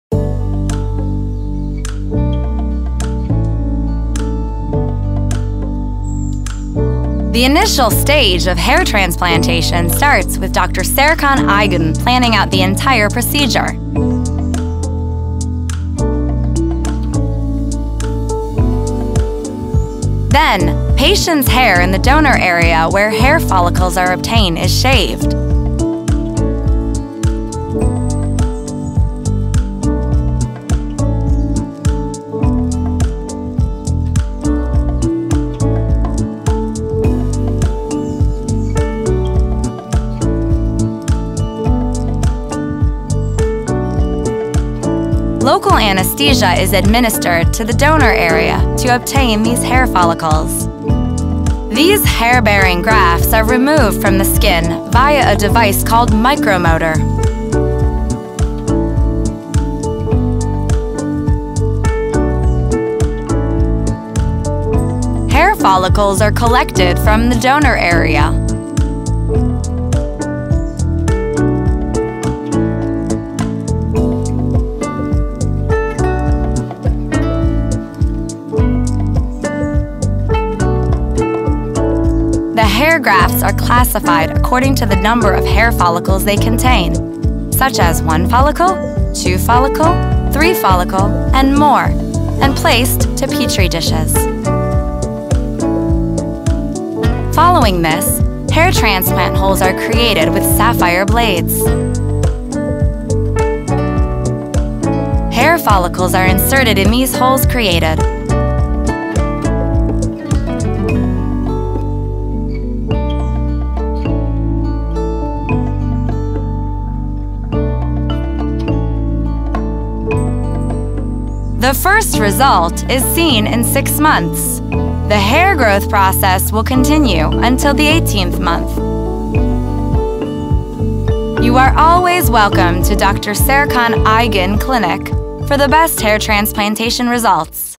Step by Step Hair Transplant Operation
Everything you wonder about the hair transplantation process, which started with the planning of Dr. Serkan Aygın, is in this animation!

How is Hair Transplantation Performed?
* The hair in the donor area where the hair will be collected is shaved and local anesthesia is applied to this area.
* Hair follicles are separated from the skin with a device called a micromotor.
* The channel is opened with sapphire tips in the area to be transplanted.
* Collected hair follicles are transplanted into the opened channels.
* And the hair transplantation process is completed.

We would be happy to welcome you in our clinic in Istanbul.

Dr. Serkan Aygın’ın planmasıyla başlayan saç ekimi süreci hakkında merak ettiğiniz her şey bu animasyonda!

Saç Ekim Operasyonu Nasıl Gerçekleşir?
* Saçların toplanacağı donör alandaki saçlar tıraş edilir ve bu alana lokal anestezi uygulanır.
* Saç kökleri mikromotor adı verilen cihaz ile deriden ayrıştırılır.
* Saç ekilecek alana safir uçlar ile kanal açılır.
* Açılan kanallara toplanan saç kökleri ekilir.
* Ve saç ekim işlemi tamamlanır.

Sizleri de İstanbul’daki kliniğimizde ağırlamaktan mutluluk duyarız.

Dr. Serkan Aygın Hair Transplant Clinic
Hair Transplantation in Turkey

To learn more information about:
1- Hair transplant Turkey: